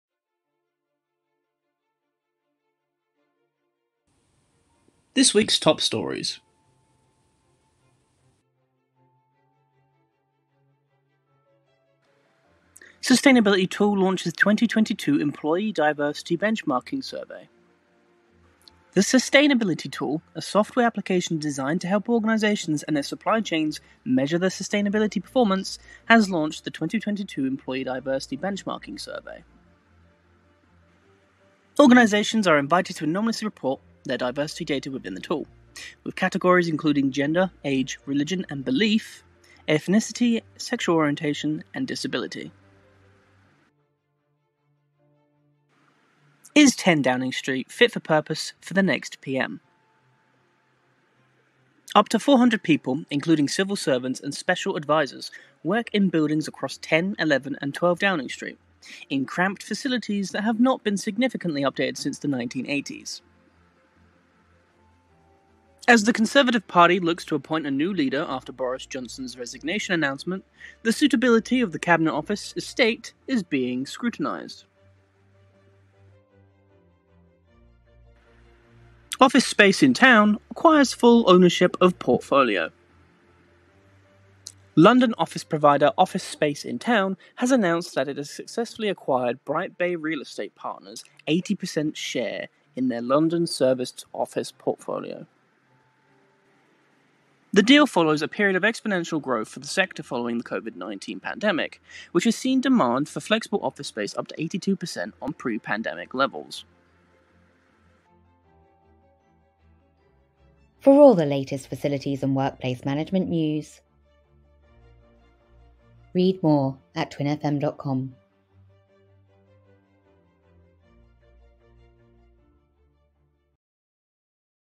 Top Stories – 15 July 2022
Sustainability Tool Launches 2022 Employee Diversity Benchmarking Survey

Is 10 Downing Street Fit for Purpose for the Next PM?

Office Space in Town Acquires Full Ownership of Portfolio